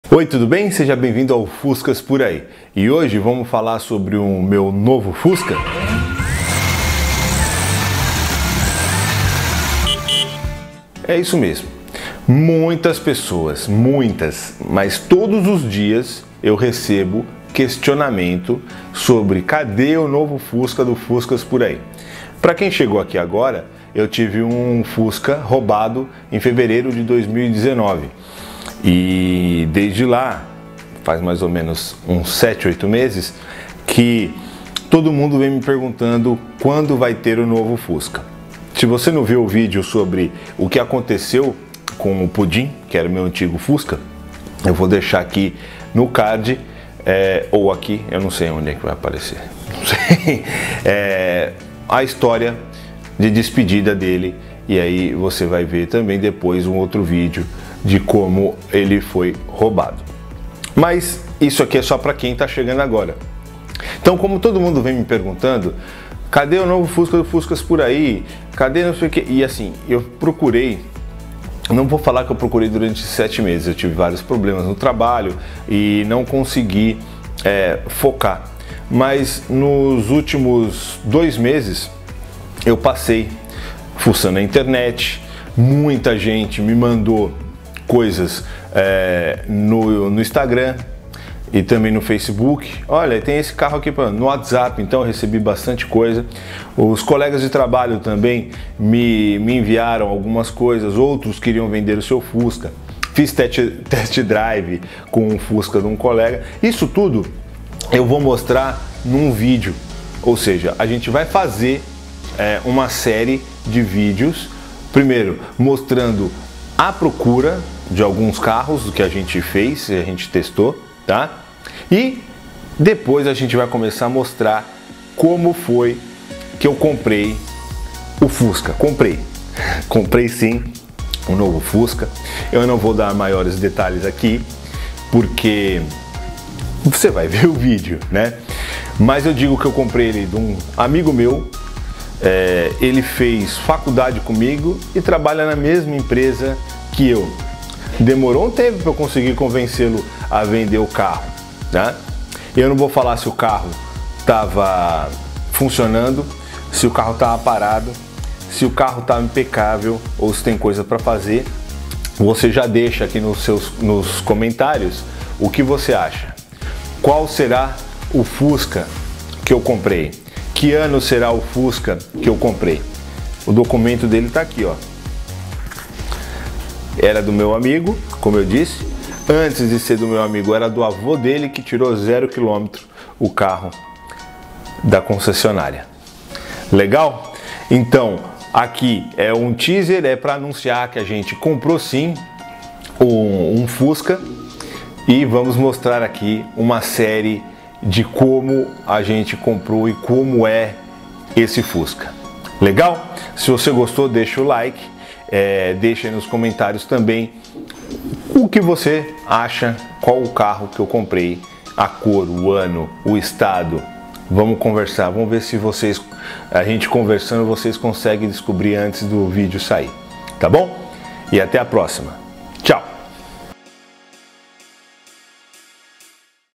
SIM, TEMOS UM NOVO FUSCA!
Depois de um período de buscas e pesquisas, encontramos um novo Fusca para o Fuscar Por Aí, saiba aí um pouco mais sobre nossa nova máquina.